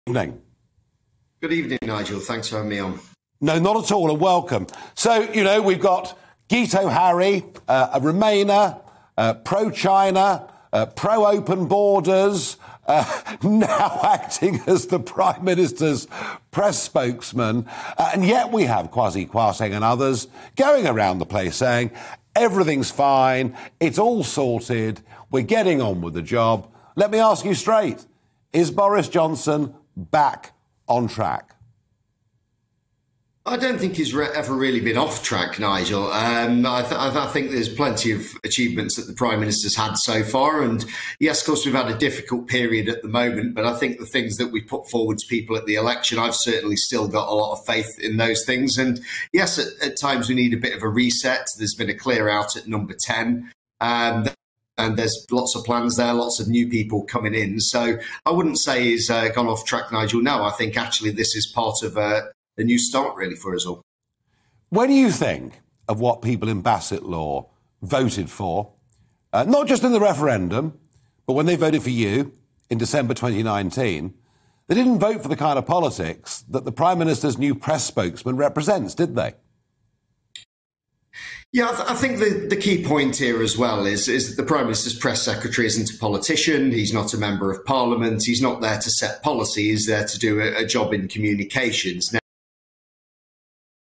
Brendan Clarke-Smith: New No.10 appointments are 'part of a new start'
‘This is part of a new start for us all’

Conservative MP Brendan Clarke-Smith discusses what new No10 appointments mean for the PM’s future.